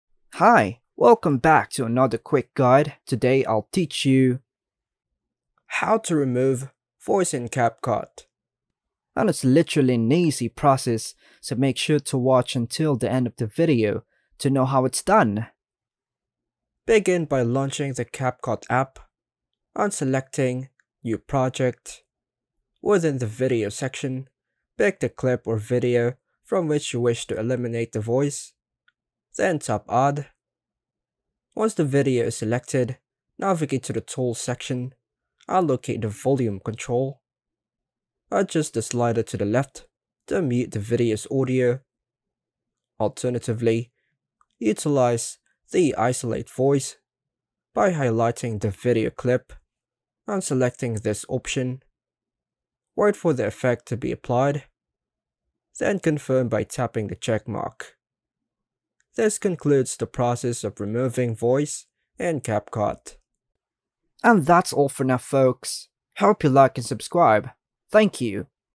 How To Remove Voice In CapCut (Easy Method)
This lesson will teach you how to remove voice in CapCut. You can use this method to edit out voices from your edits in CapCut.